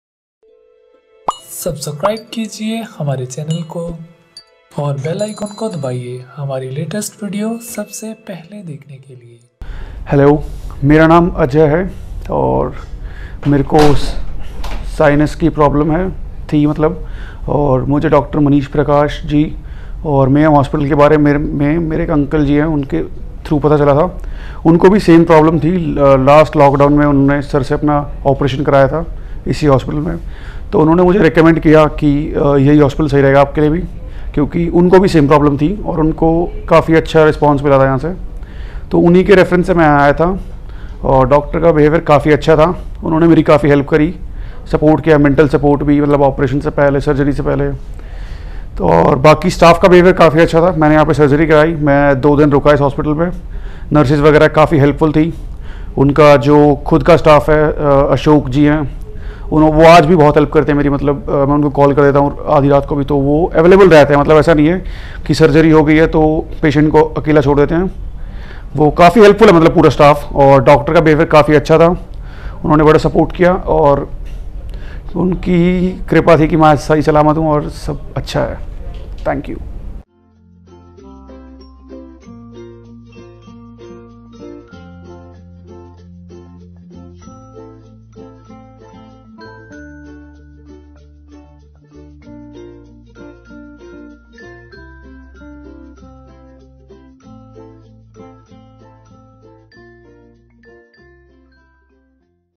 Patient Feedback after Sinus Surgery at Mayom Hospital Gurugram by Dr. Manish Prakash
Endoscopic sinus surgery or complicated nose surgery is performed by ENT Specialists in Gurugram. Visit him today if you are facing any difficulty with your nose.